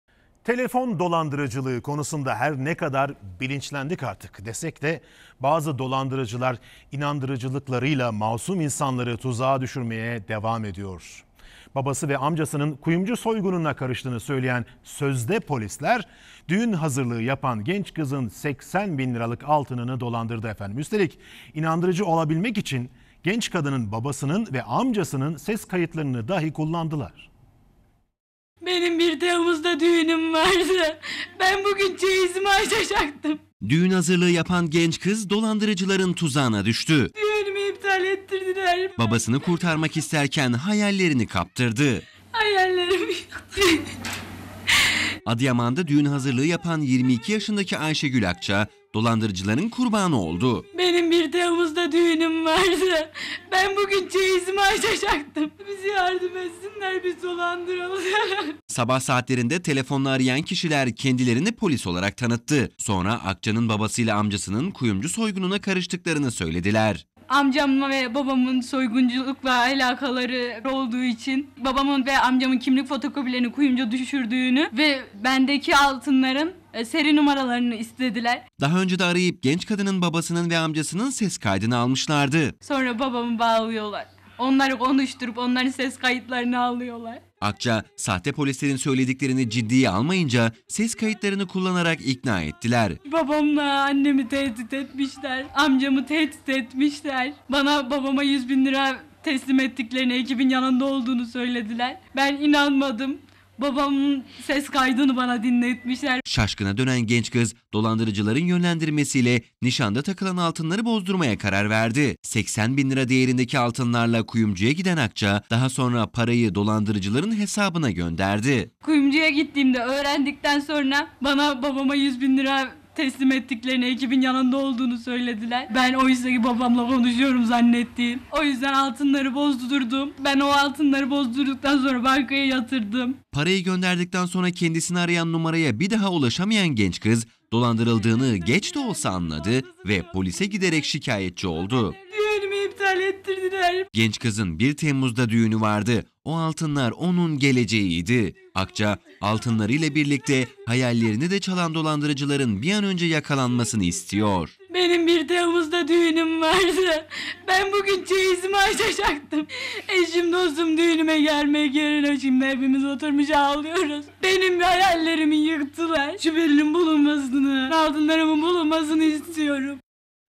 Adıyaman'daki montajlı dolandırıcılık bu kadar da olmaz dedirtti
Adıyaman'daki montajlı dolandırıcılık bu kadar da olmaz dedirtti Babasının ve amcasının seslerini montajlayan dolandırıcılar, genç kızı kandırarak altın paralarını aldı Düğünü iptal olan genç kız ve ailesi göz yaşlarına boğuldu